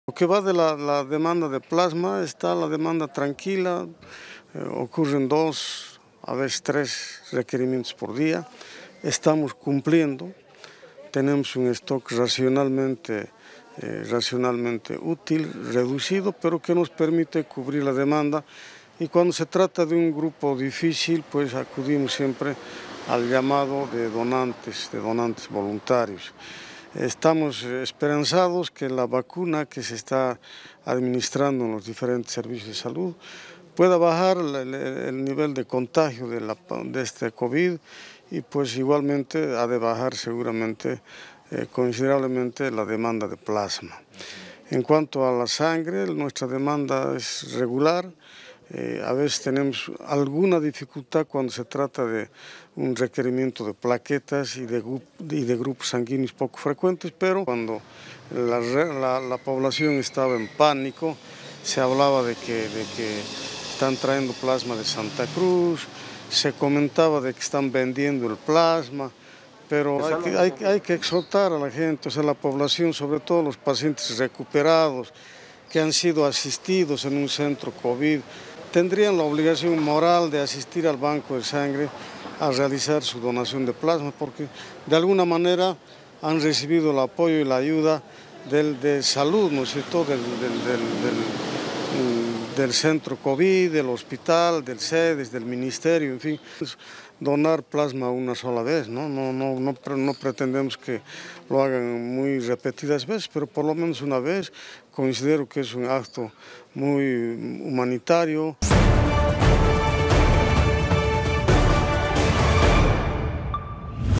TODOS LOS MARTES EL BANCO DE SANGRE ES FUMIGADO POR PRECIPICIÓN ANTE EL COVID - 19
TODOS LOS MARTES EL BANCO DE SANGRE ES FUMIGADO POR PRECUACION ANTE EL COVID, SU DIRECTOR AFIRMA QUE SE ATIENDE LOS REQUERIMIENTOS DE PLASMA PARA LOS ENFERMOS MOTIVANDO A QUE LA GENTE CONTINUE DANDO PLASMA EN ESTE PANDEMIA